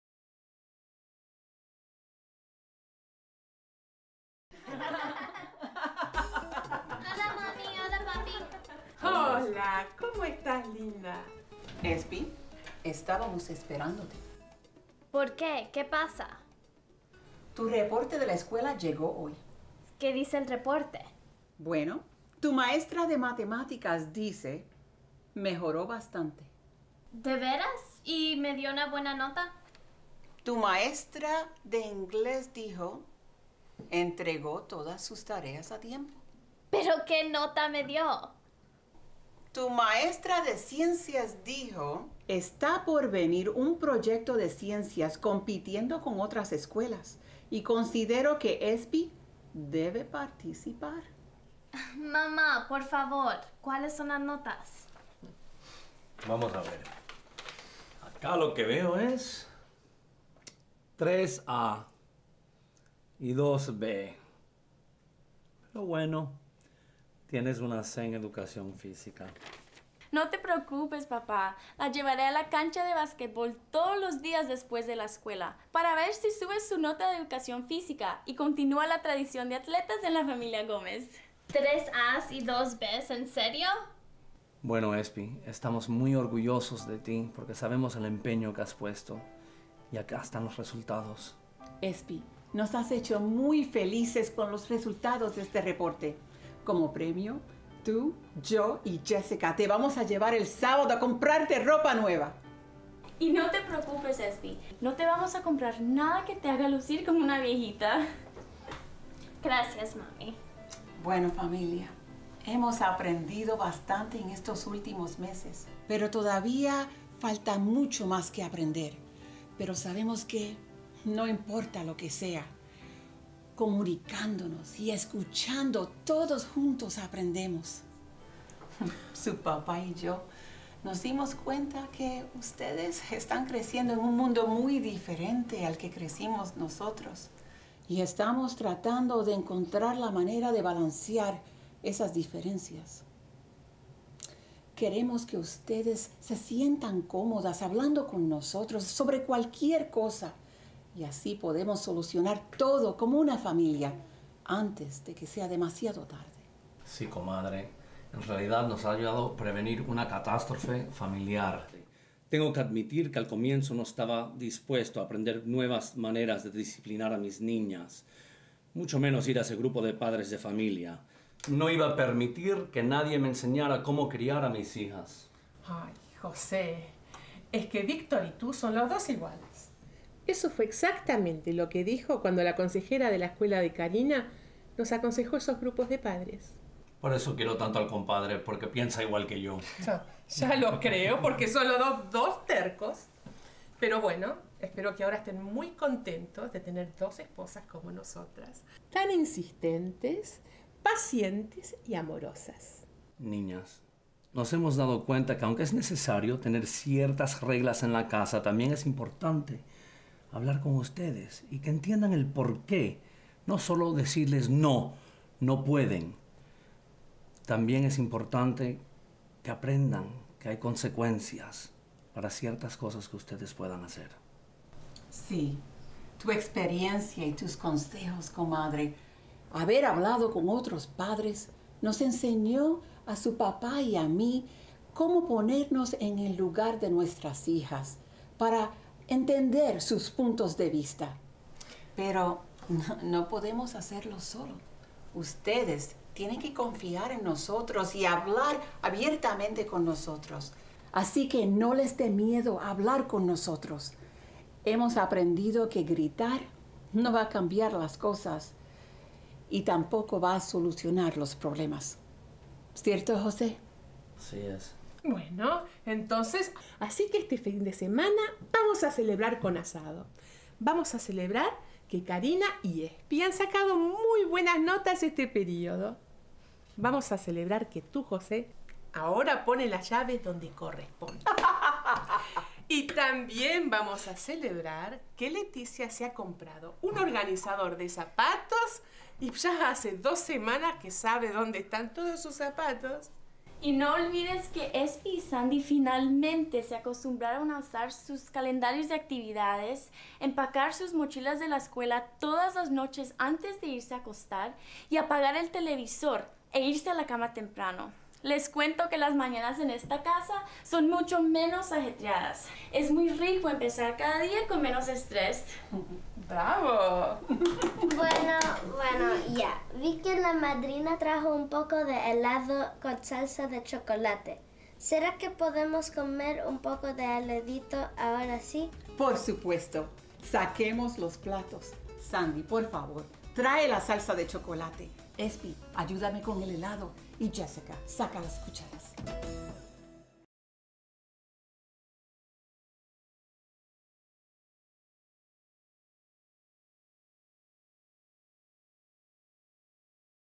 Resumen de los temas y resolución (Escena 8)
En este video, unos padres celebran los éxitos de sus hijas adolescentes, tanto en la escuela como en el hogar. Para más información sobre cómo mejorar la comunicación entre padres e hijos, visite